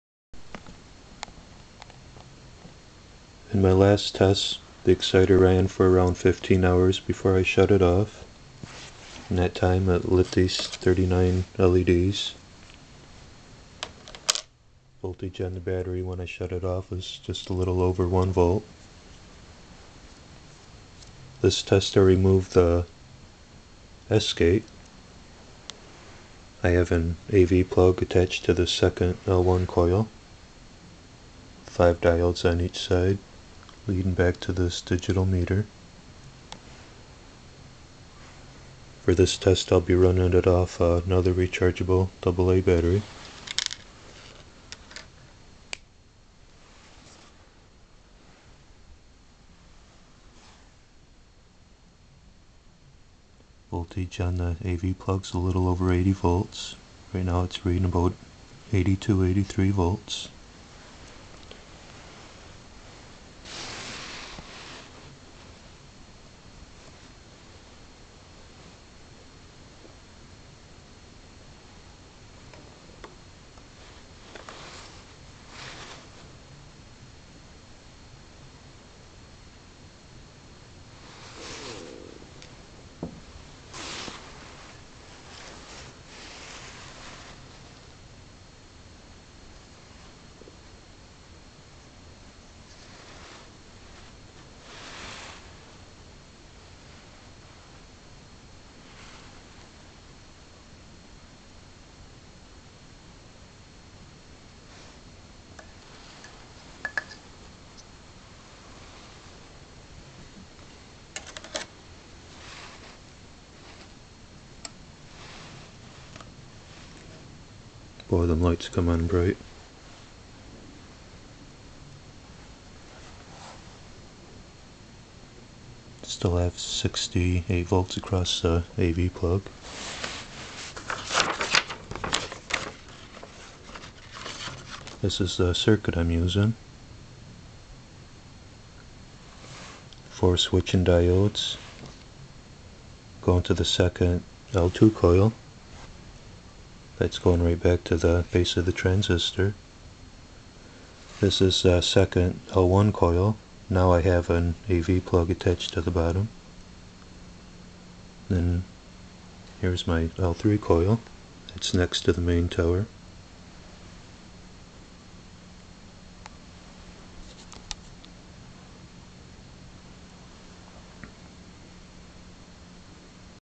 Exciter With Second Coil And Tower Update.
My last test I let the Exciter run for 15 hours before turning it off.
The voltage in the battery was 1.1v after running 15 hours and lighting the 39 led's.

For this test I removed the Sgate and added an AV plug to the bottom of the second tower.

More info at the Energetic forum or overunity.com